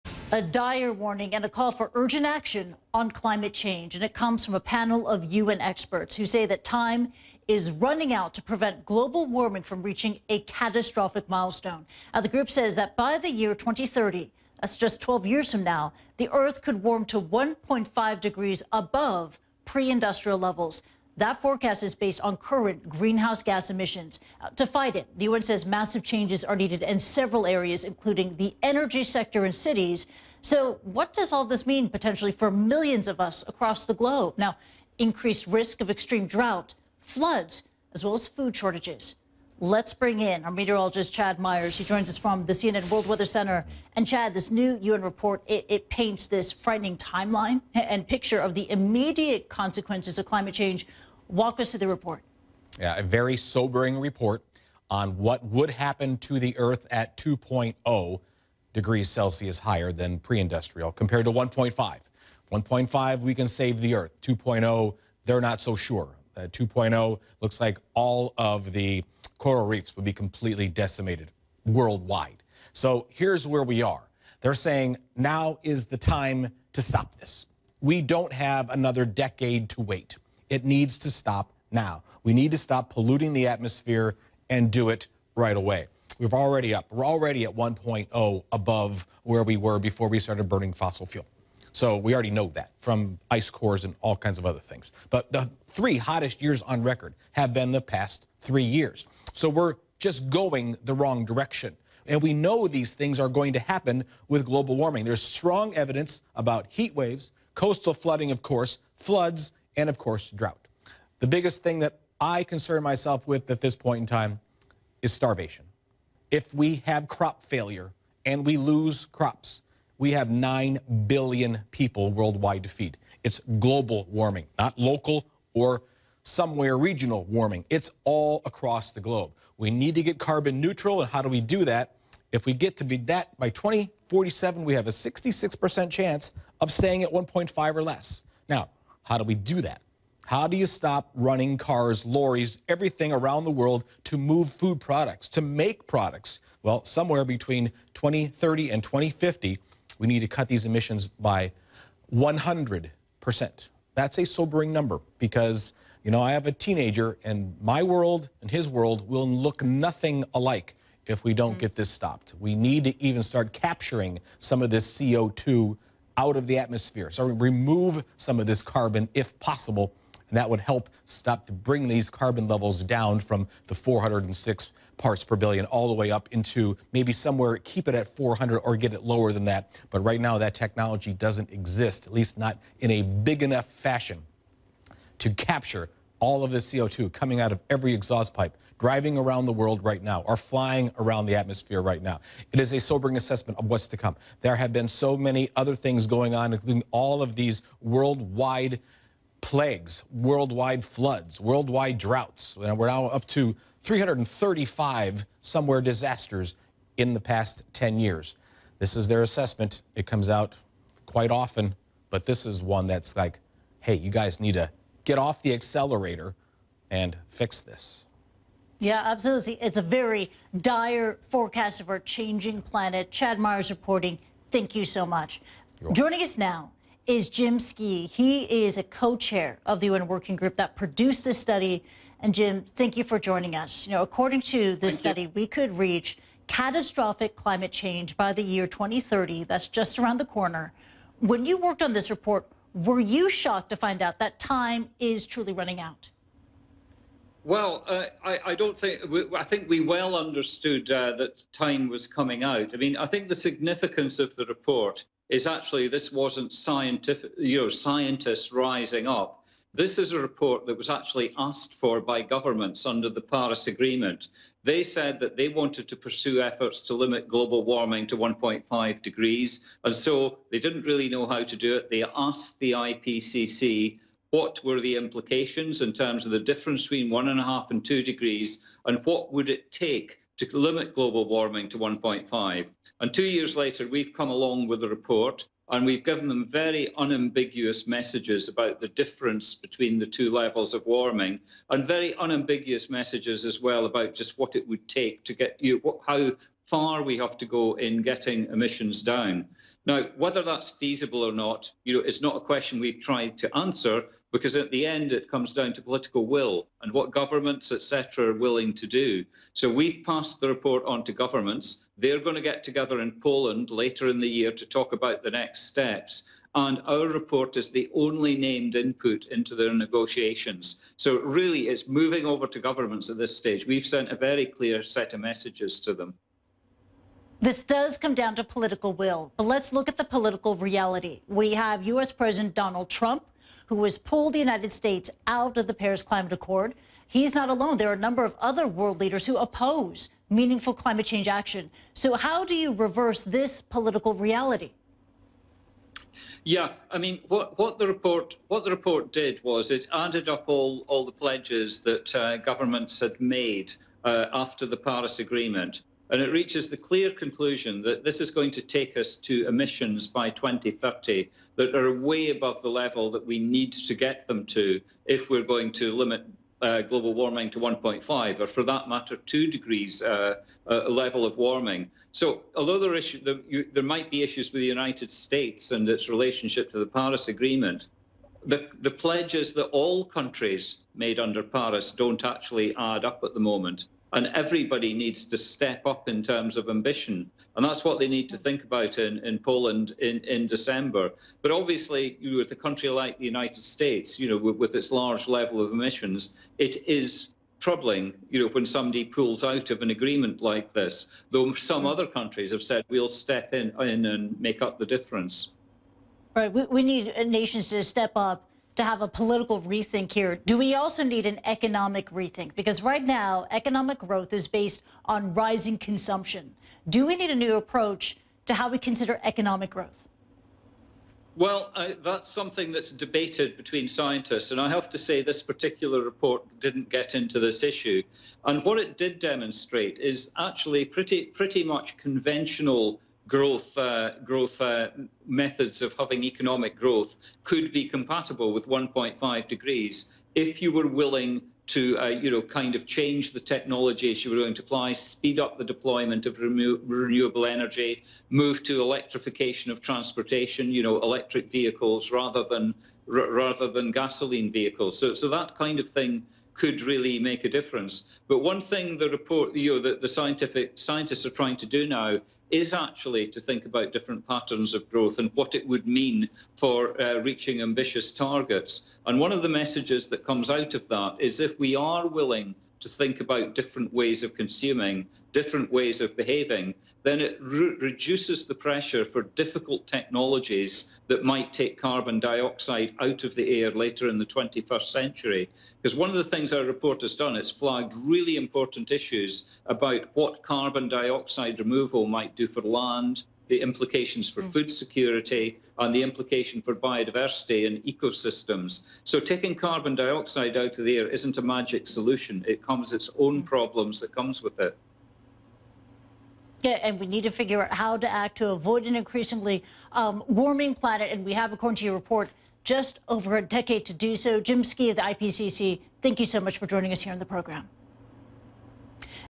UN: Earth has 12 years to avert climate change catastrophe
According to a new UN report, we have just over 10 years to get climate change under control. CNN's Kristie Lu Stout talks to meteorologist Chad Myers and Jim Skea, IPCC co-chair behind the study, about the landmark report.